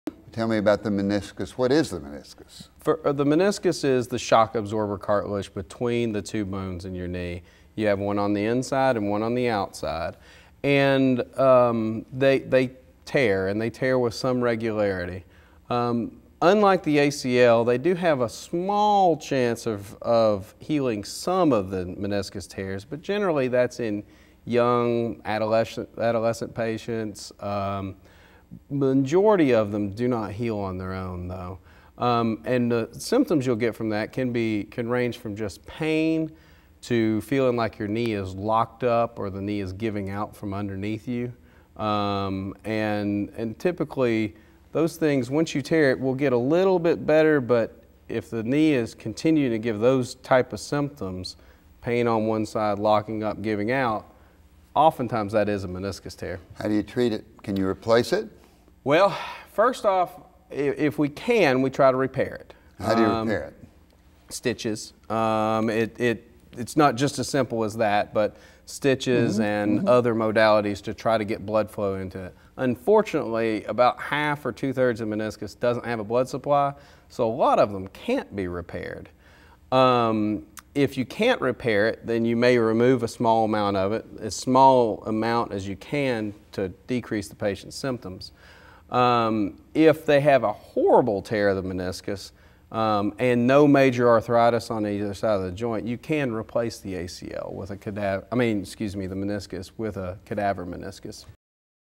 Meniscus Tear (Knee Injury) Explained
Orthopedic surgeon, Dr. Chris Shaver explains a specific knee injury called the meniscus tear and goes on to explain the various options in which it can be treated.